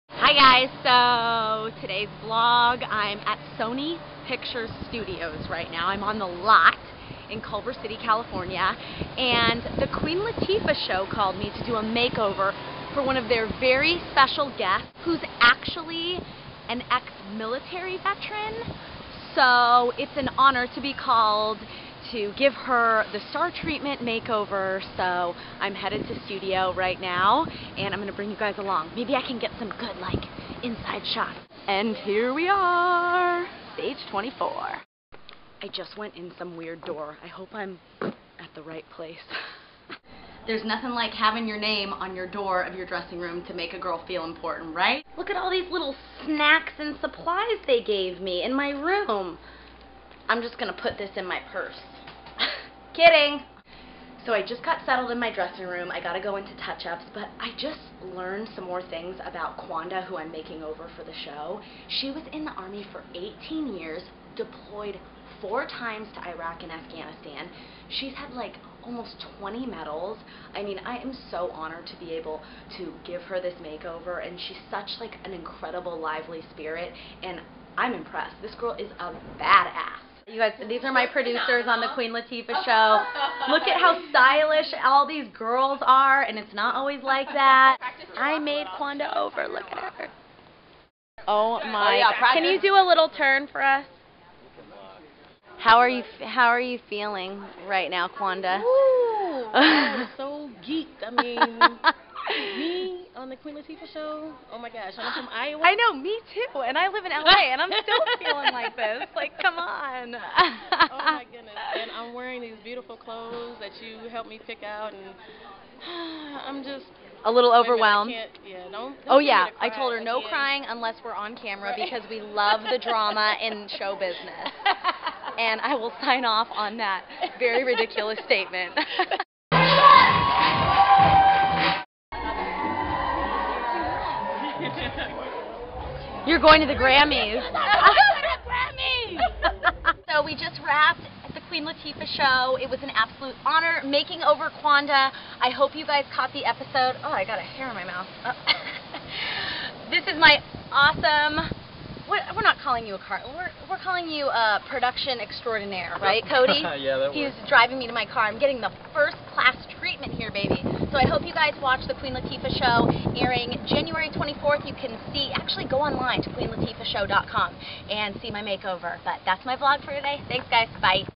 My Queen Latifah show Makeover!!!!
Hi Friends!!
I was honored when The Queen Latifah Show called me to do a makeover for one of their VERY deserving viewers. Check out this weeks vlog to get in on all the makeover magic action! Please also LIKE if you want me to do more vlogs like this!!! Also SUBB and FAVE if you really really love me. haha!! LOVE: Linds